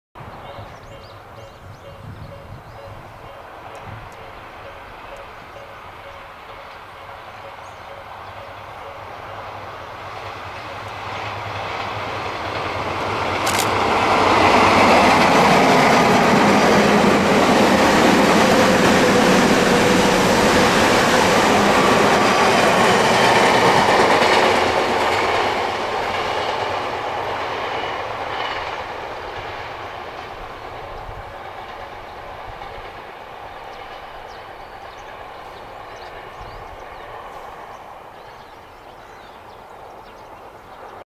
東武30000系31401F+東武10030系11256F・東武30000系31402F+東武10030系11262F 久喜～鷲宮通過
2010/3/23 東武伊勢崎線 久喜～鷲宮にて